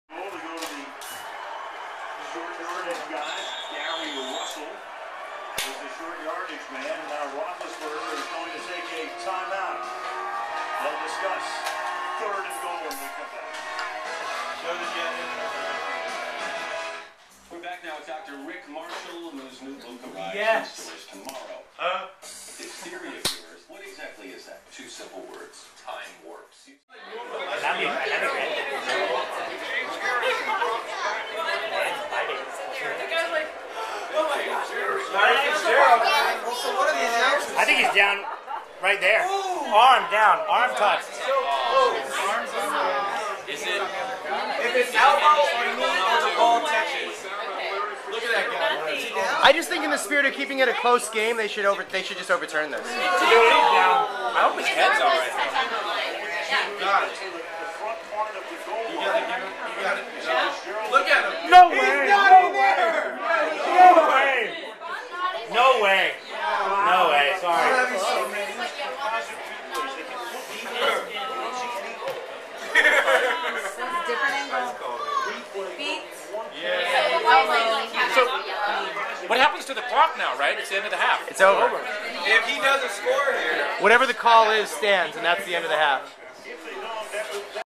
Super Bowl Party | 01 Feb 2009
Over 30 people at my house for a Super Bowl Party.